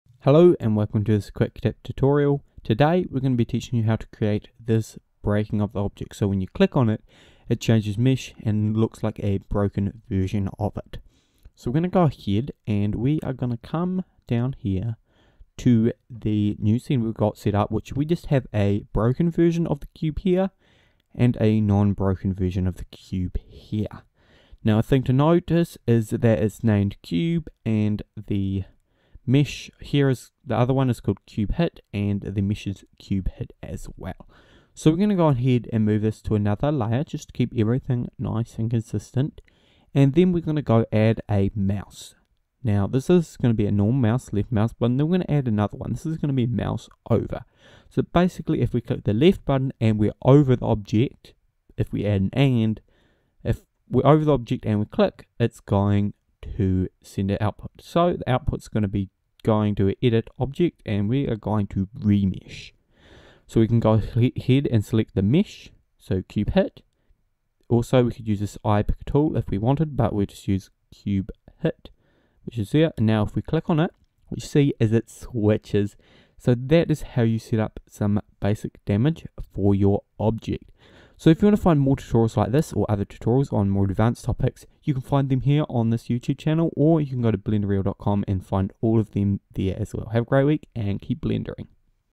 Damaging object mesh - BGE Tutorial
let's learn how to fake Damaging the object mesh